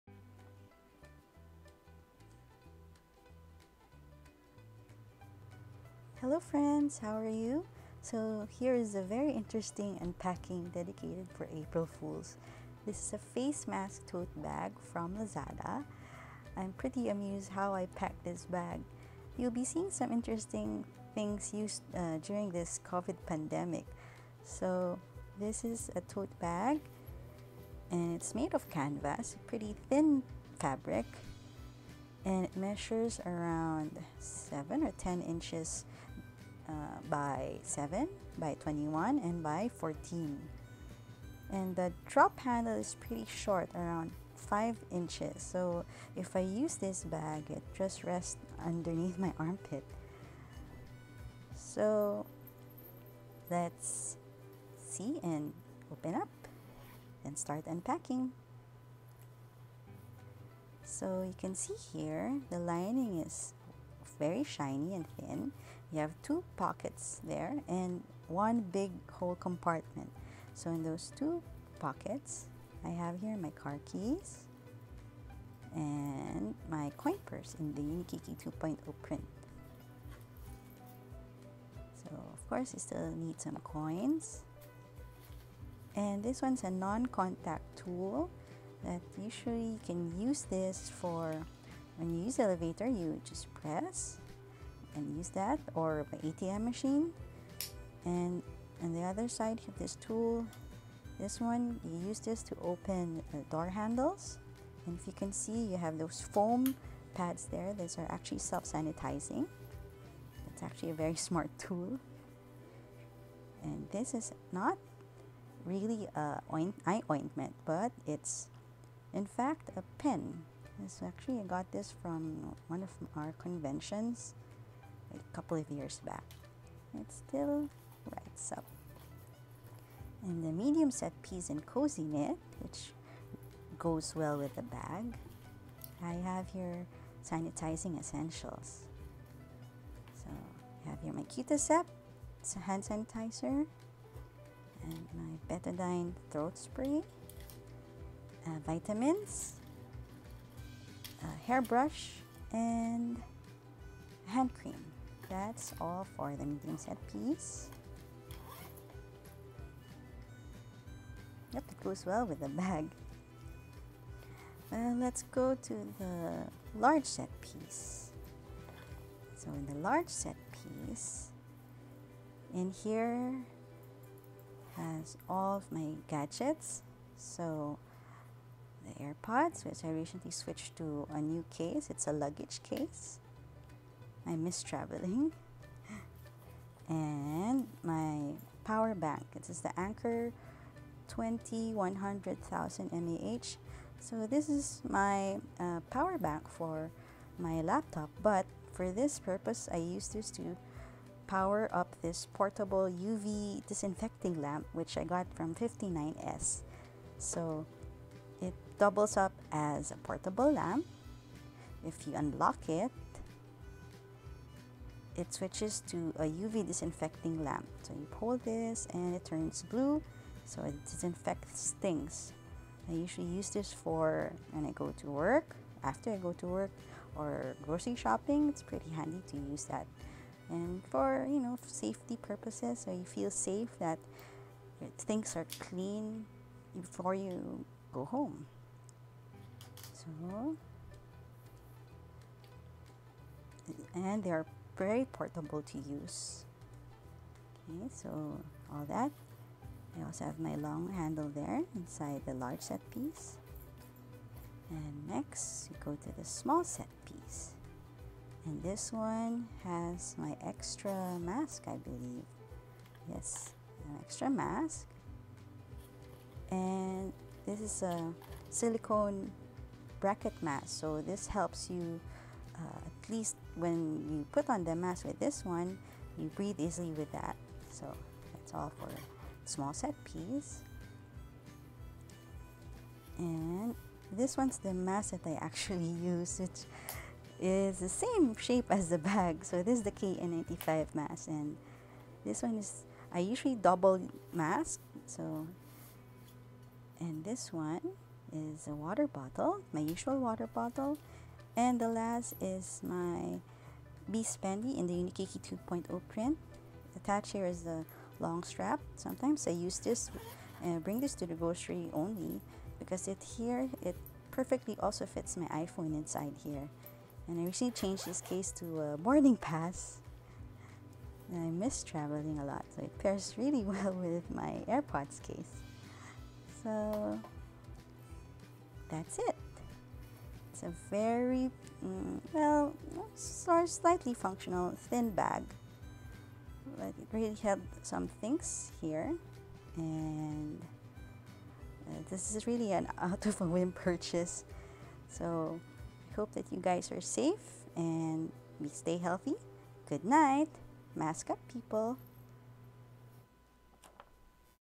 Unpack with me (April fools day edition) : Face Mask Tote bag feat. Jujube Be Set in Cozy Knit
Unpacking this face mask tote for fun! Interesting things are inside this bag -specifically anti-COVID related gadgets. I made this video just for me to unwind. Enjoy the video! Stay safe and mask up!